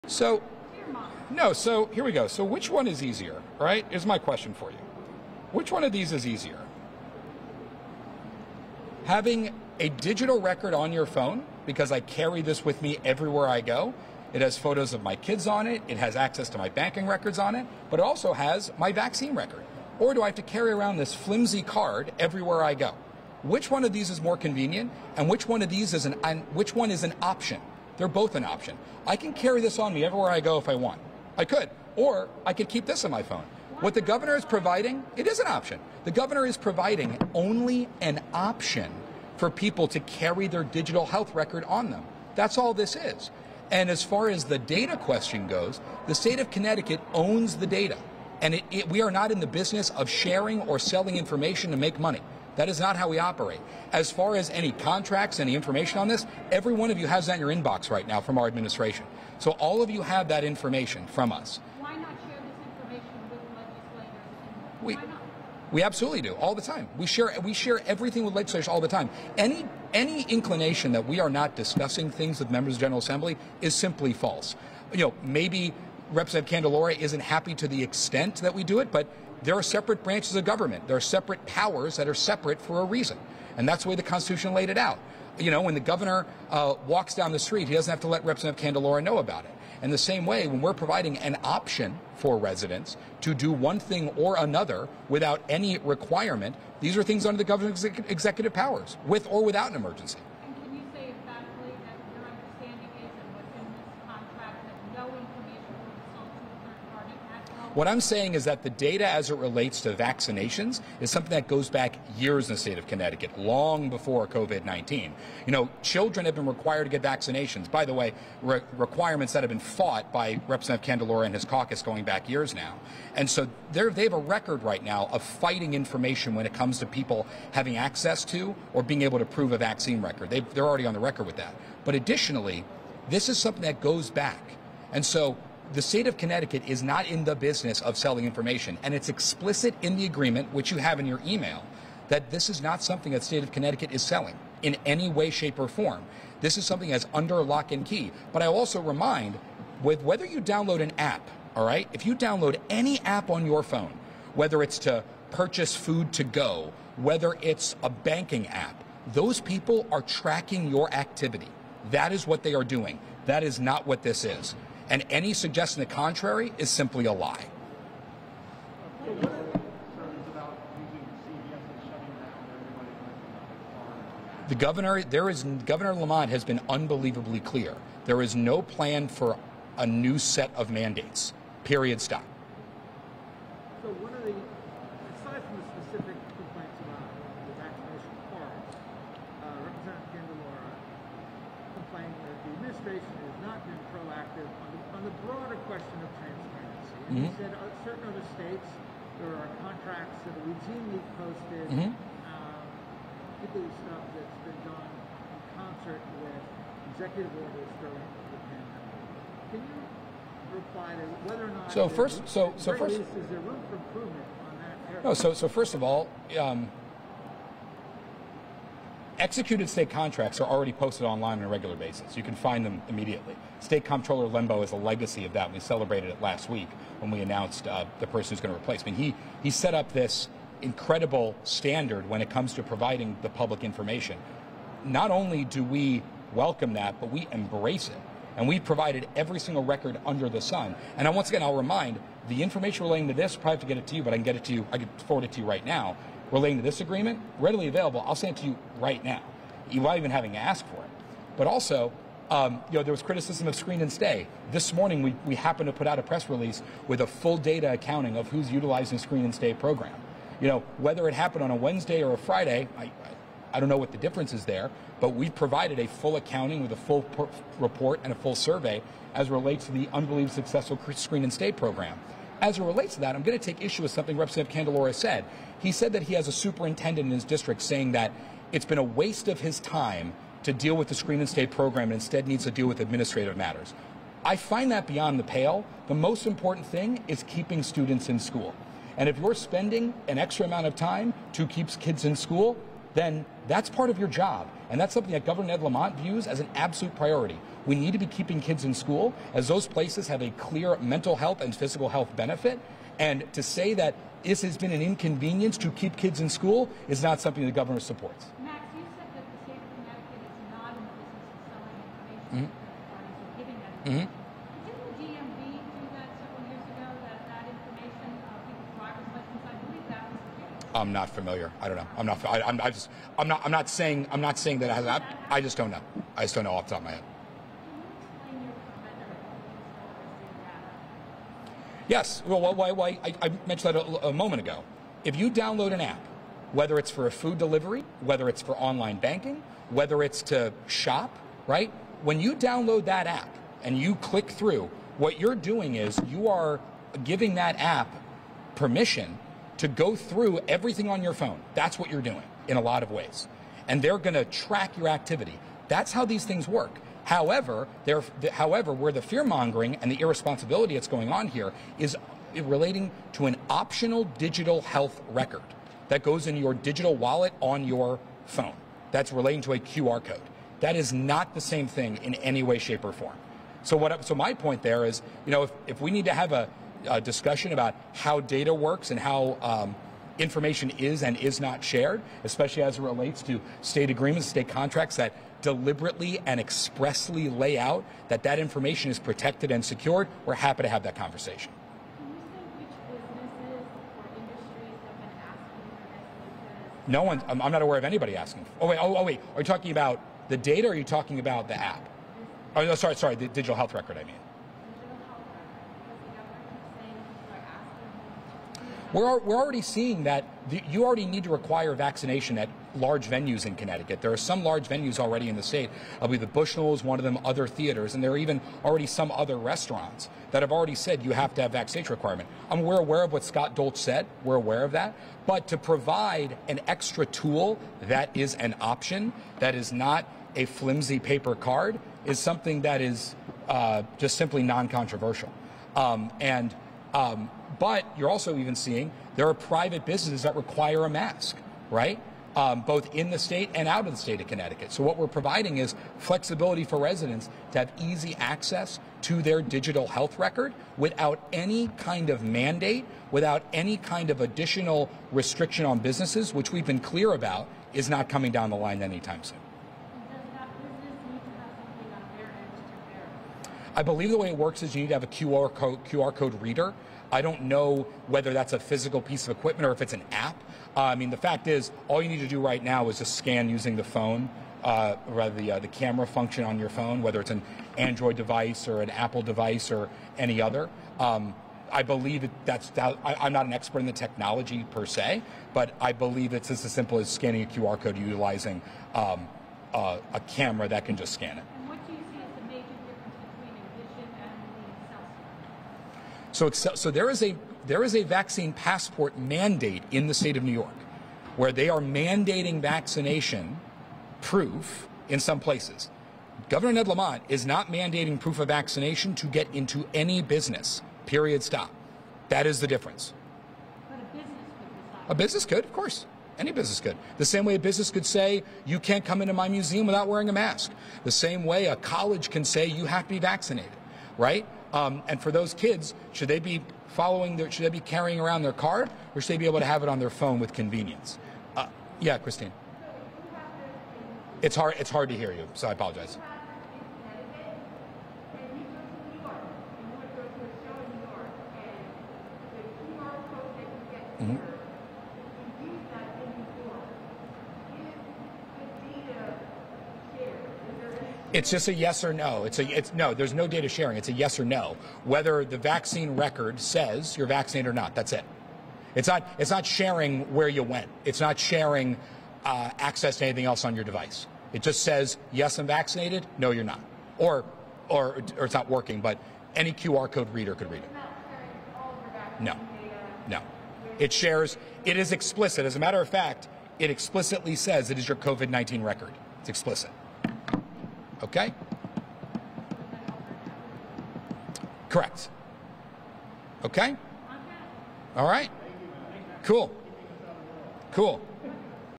NEWS CONFERENCE: Gov. Lamont's office response to Republican questions about digital vaccine
Gov. Ned Lamont's director of communications, Max Reiss, responded to questions posed by state Republicans about the governor's digital vaccine passport program.